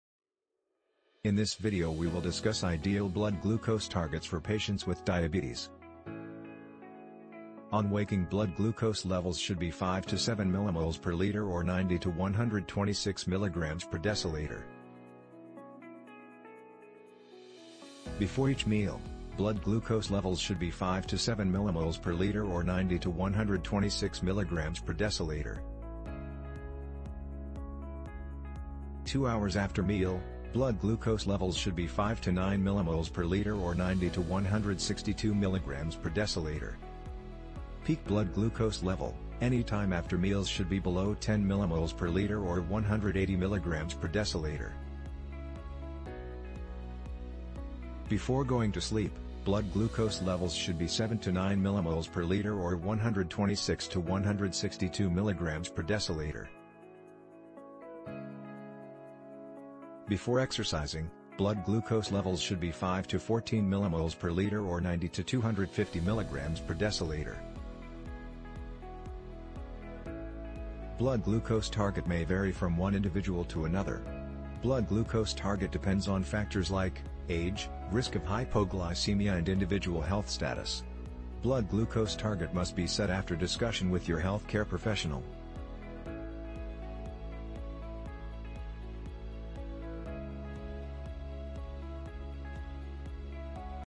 Ideal blood glucose levels in individuals with diabetes mellitus
What is the ideal blood glucose level in individuals with diabetes mellitus?